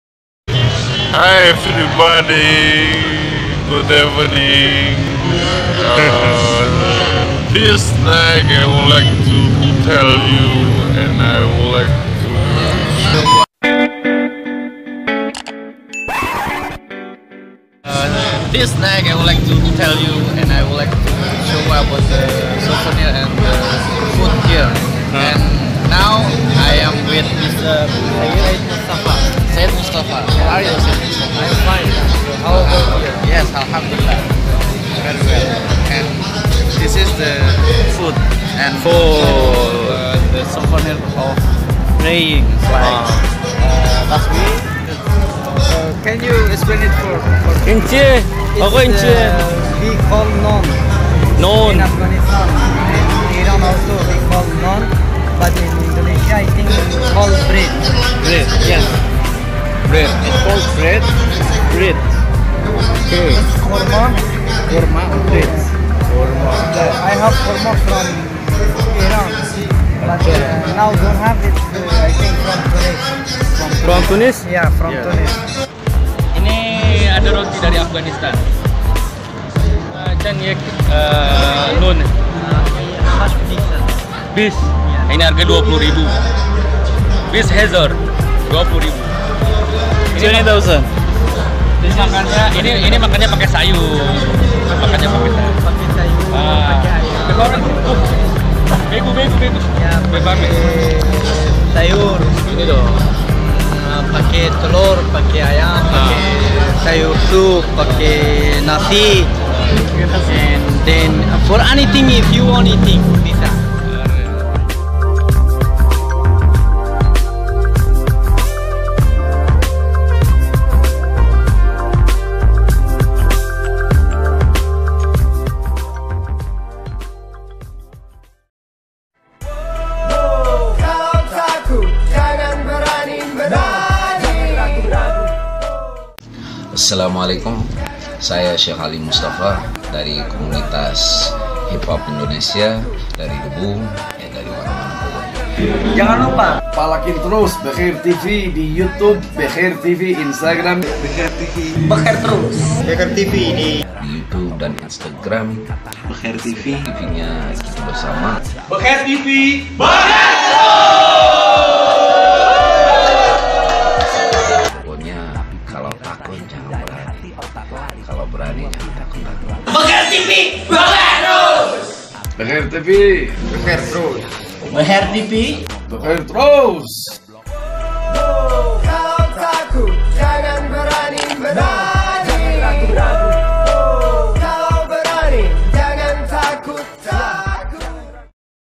Hunting Makanan Timur Tengah - Roti Naan Khas Afghanistan
Ada banyak makanan khas timur tengah, kali ini saya memposting tentang makanan khas dari Afghanistan dan Iran yang bernama "Naan" atau roti Nun.